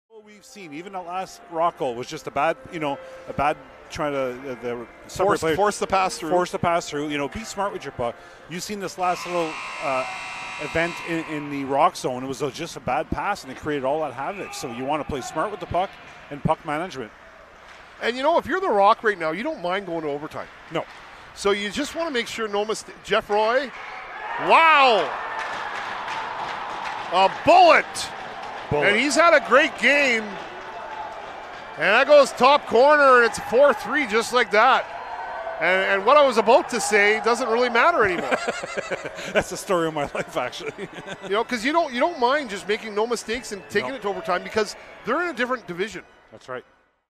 Cubs at Rock
Greater Sudbury 6 at Timmins 3
The Greater Sudbury Cubs capped off a three-game road trip by getting two goals from Cameron Shanks as part of a 6-3 victory over the Timmins Rock in a Family Day Northern Ontario Junior Hockey League matinee Monday at McIntyre Arena.
After controlling a draw, the visitors worked it across to Carter Geoffroy who proceeded to move in from the point and blast a shot from the top of the right circle that flew into the back of the net, for the eventual game-winner.
Play by play: Shawn McArthur
Colour commentary: Jason Kelly